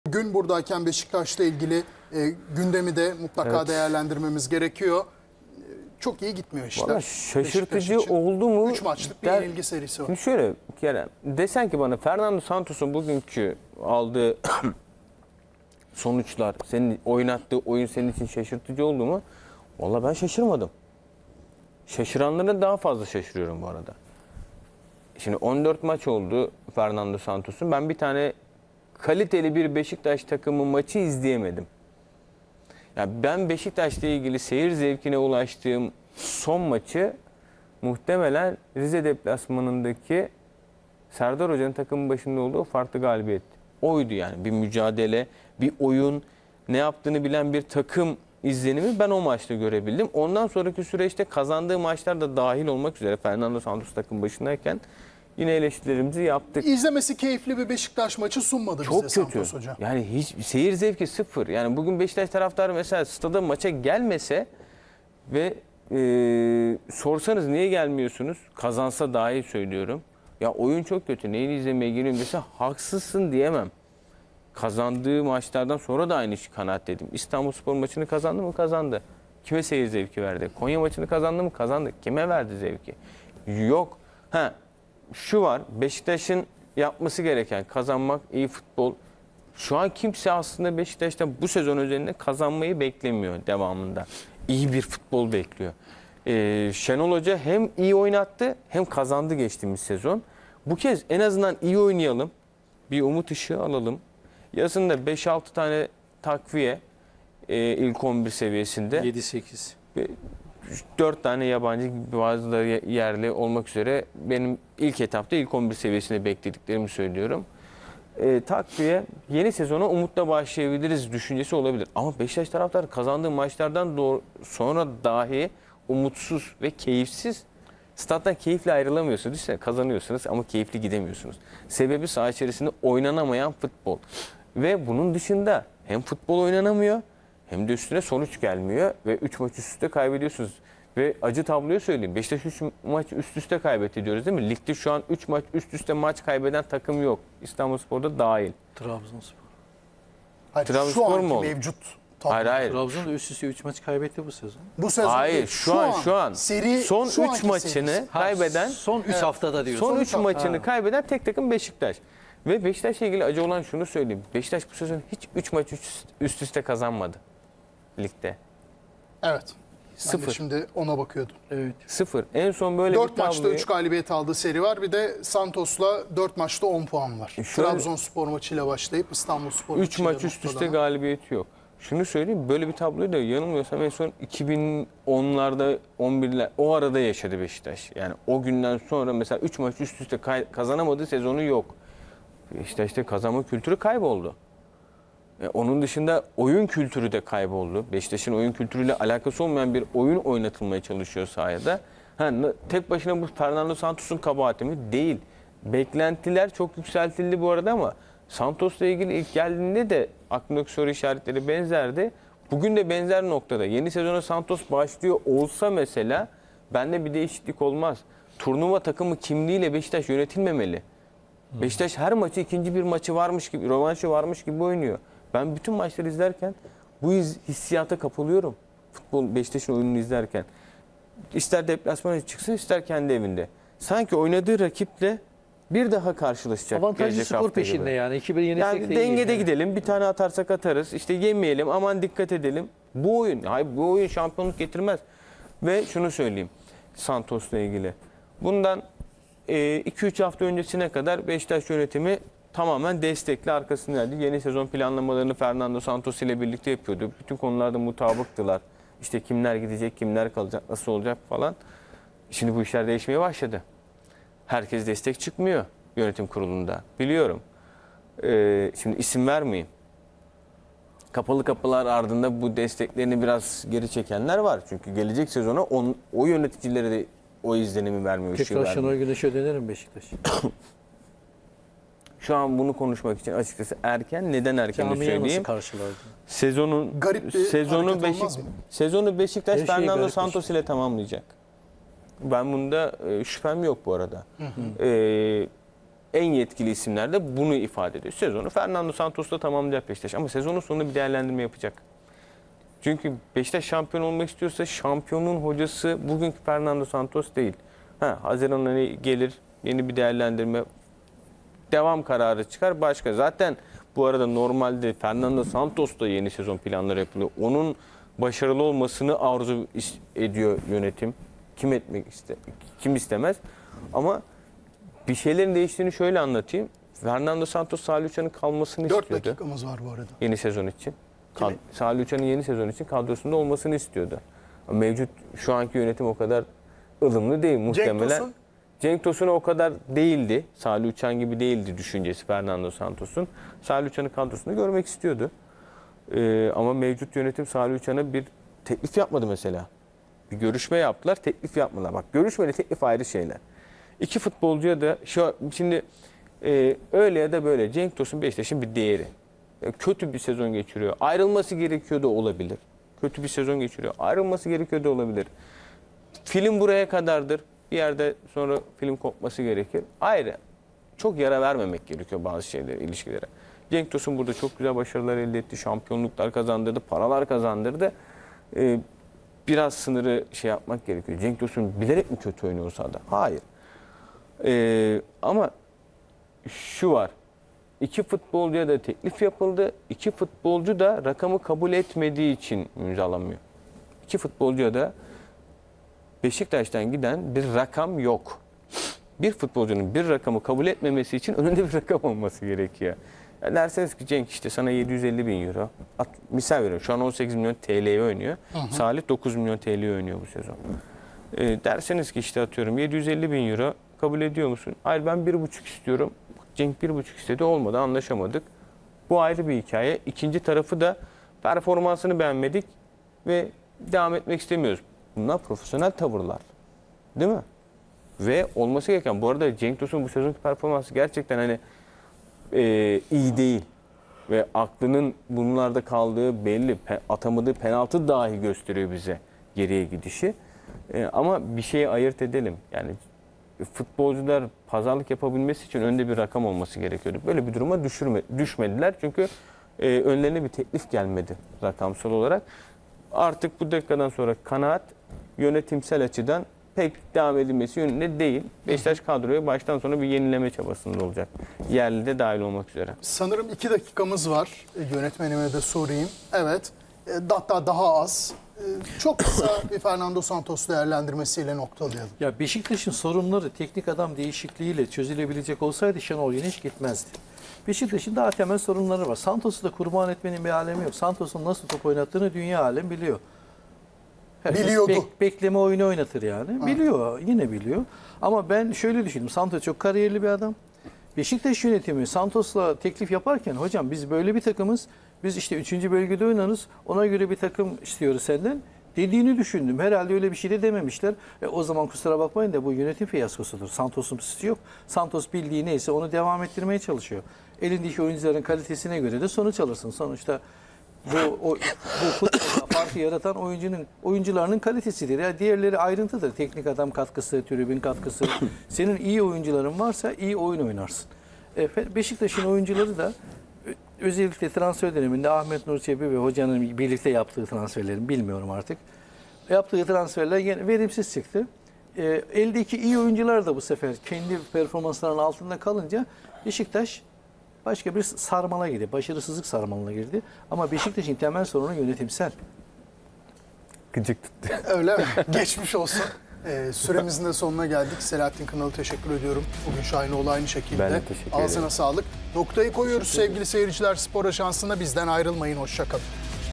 Ogün Şahinoğlu: "Beşiktaş Kazandığı Maçta Bile Taraftarına Oyun Zevki Vermiyor!" / A Spor
Ogün Şahinoğlu: "Beşiktaş Kazandığı Maçta Bile Taraftarına Oyun Zevki Vermiyor!" / A Spor / Spor Ajansı / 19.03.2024